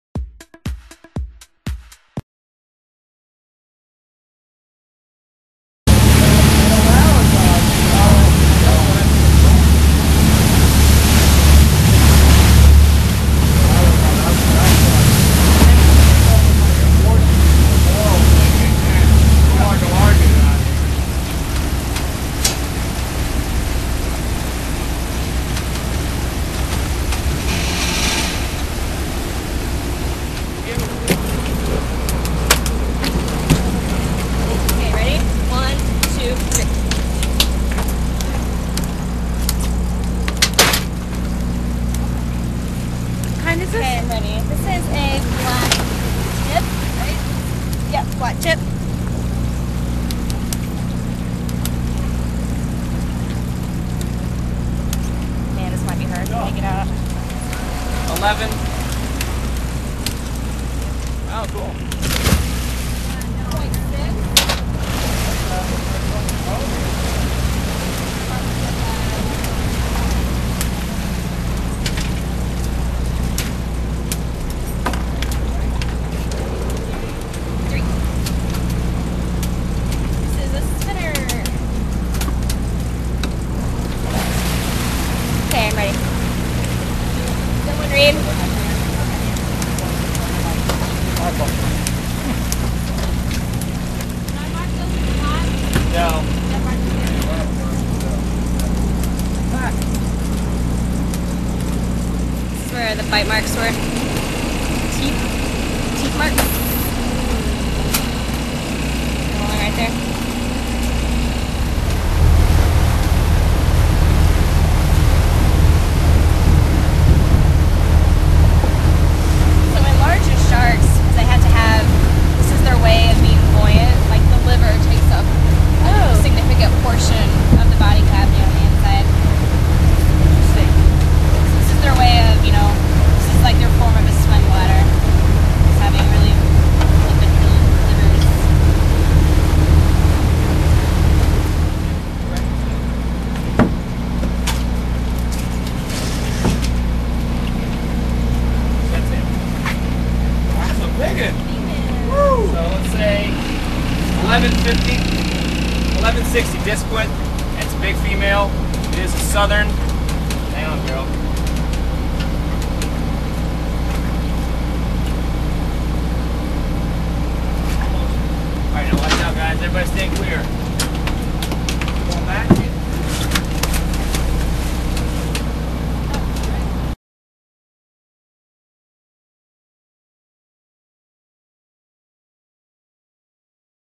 Gulf Coast Shark Survey
Scientists with the Dauphin Island Sea Lab are surveying the shark populations in the more shallow waters off Alabama. It's an attempt to track the population and keep a watch on any changes in the costal ecosystem. The Press-Register them Wednesday, Sept. 17, 2008, as they caught nearly 40 sharks including a 9-foot great hammerhead.